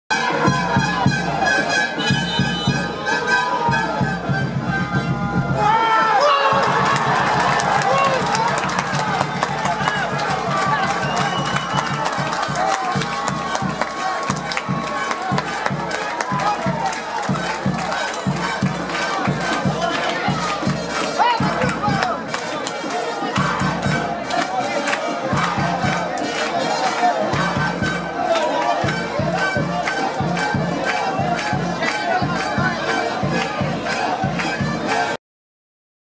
COPA PERÚ: Gol de Laure Sur Chancay gol Molina
Gol de Molina para Defensor Laure Sur de Chancay frente a Juventud Huaripache Video de Felipe Carlos